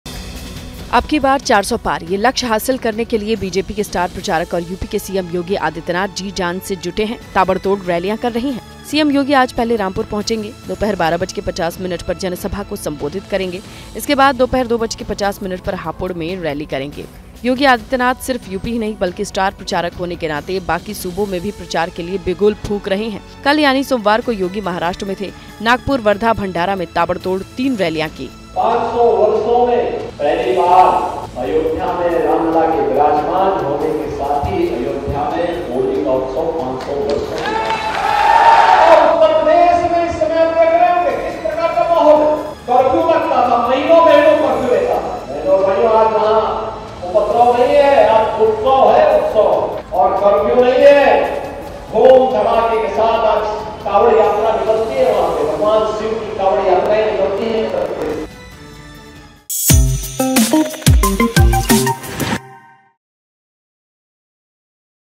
Loksabha Election 2024: लोकसभा चुनाव प्रचार के लिए Uttar Pradesh में CM Yogi, जानिए पूरा कार्यक्रम
News: लोकसभा चुनाव के लिए यूपी के सीएम योगी आदित्यनाथ आज भी धुआंधार रैलियां करेंगे...कल महाराष्ट्र में हुंकार भरने के बाद योगी आज अपने ही सूबे यानी यूपी में हुंकार भरेंगे